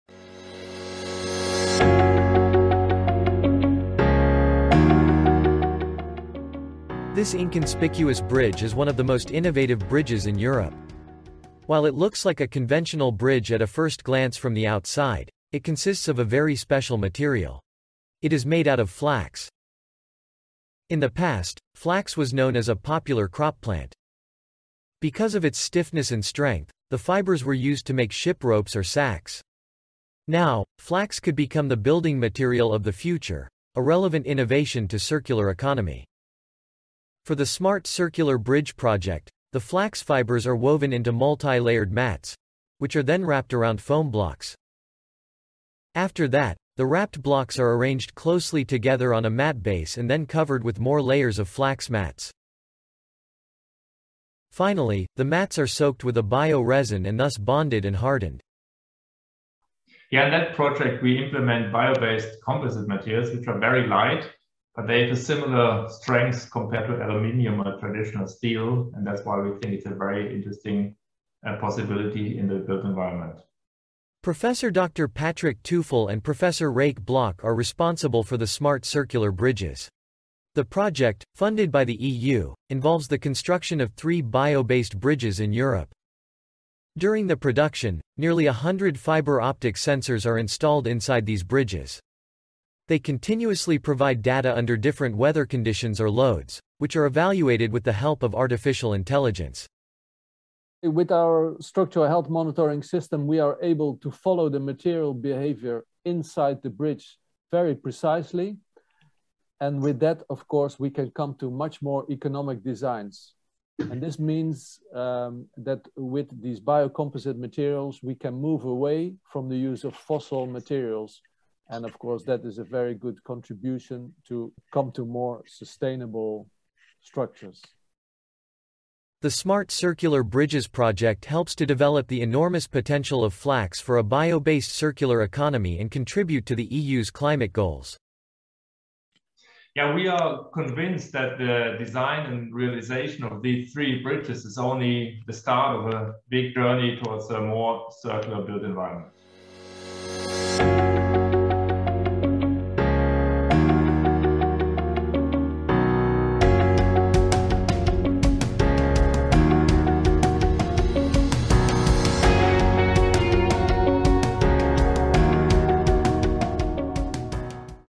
Smart Circular Bridge: A High-tech bridge built with flax | General Information | English
Innovations for climate protection and circular economy: The first "Smart Circular Bridge" with a span of 15 metres has now been realised at the Floriade, international horticulture exhibition in Almere, Netherlands. It will be opened April 22.